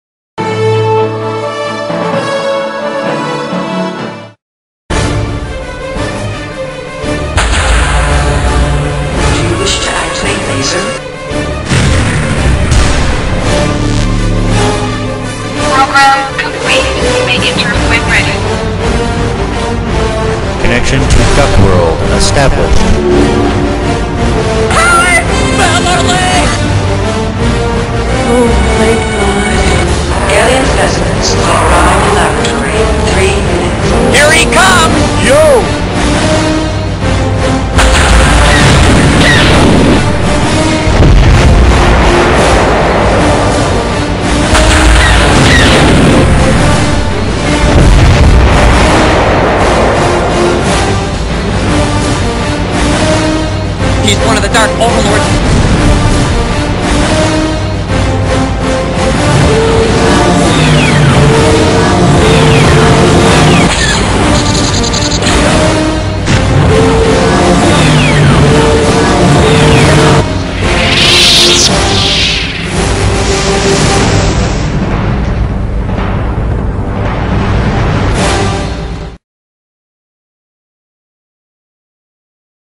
howard the duck 2 movie trailer
this is a fan-made movie trailer for a movie that was never made and probably never will be,george lucas might change his mind. NOT!

P.S , i have found the creator of this clip, please visit his channel and check out his VERY well made animation!